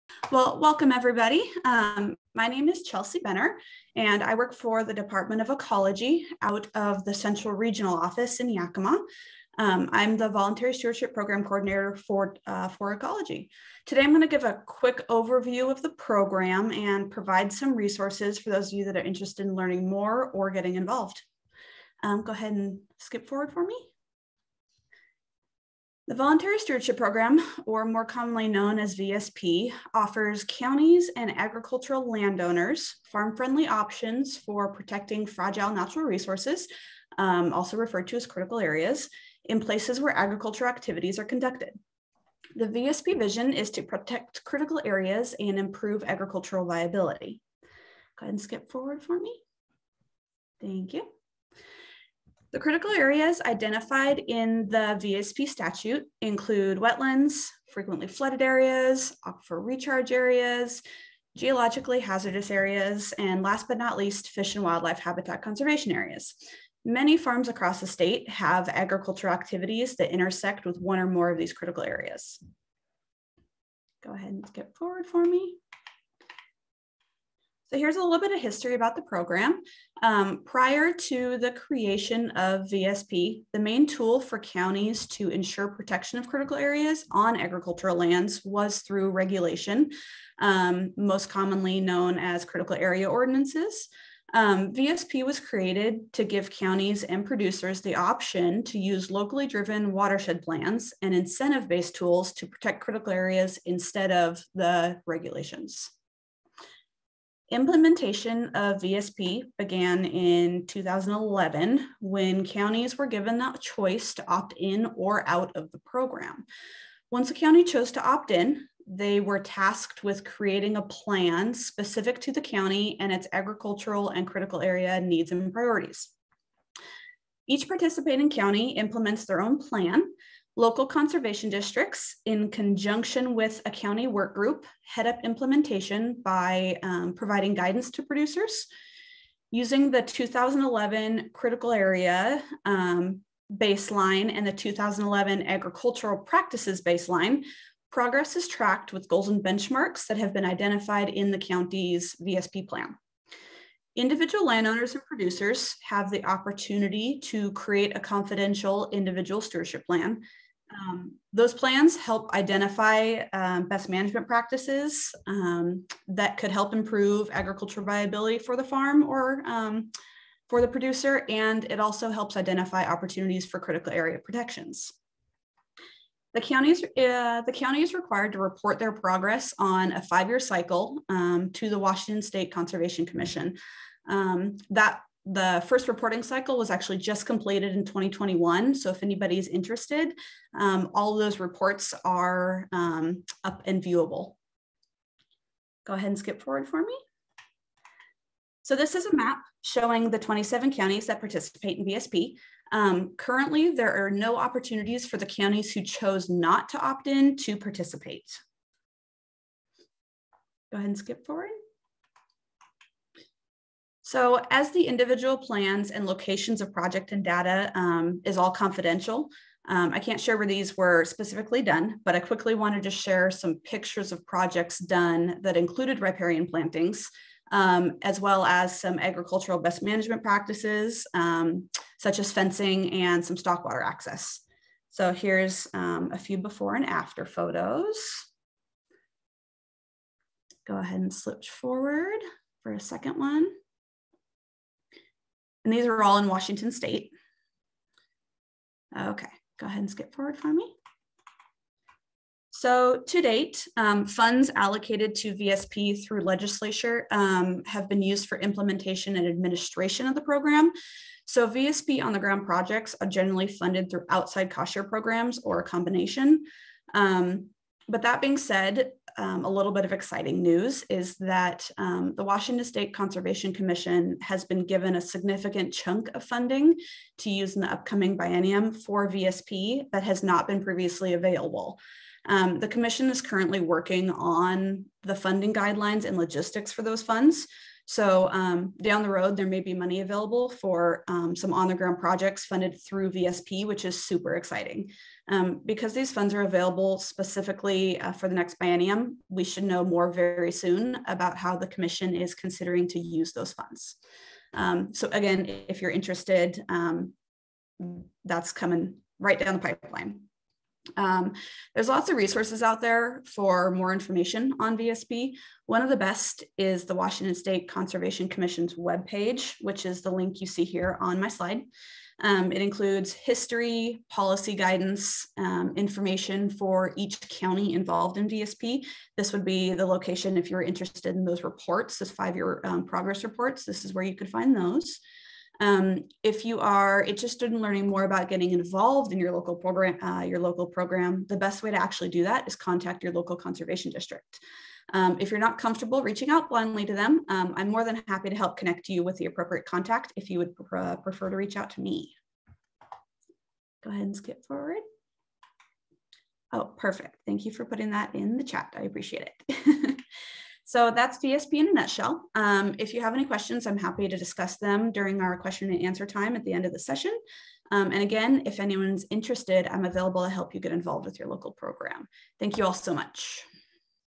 Voluntary Stewardship Program
CHELSEA BENNER - Washington Department of Ecology, Voluntary Stewardship Coordinator

Chelsea has been with Ecology for two and a half years, including prior experience with Kittitas County as a land use planner.

Presentation Overview
-What is the Voluntary Stewardship Program (VSP)
-How you can get involved

Thanks to Tim Poppleton of the Department of Ecology for processing this video.